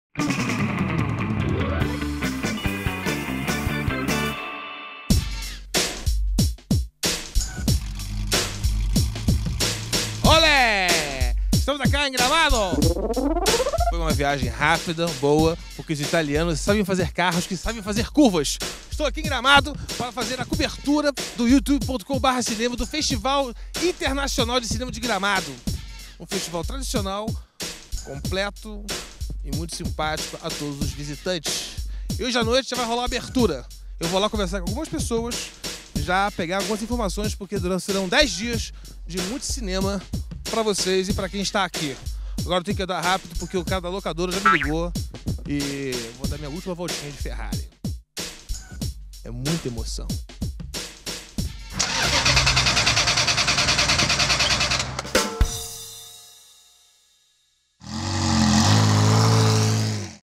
Festival de Gramado - Chegamos com glamour!
Paulo Tiefenthaler, gentilmente cedido pela MTV, chega ao Festival de Cinema de Gramado em grande estilo.

Agradecimentos:
Super Carros Gramado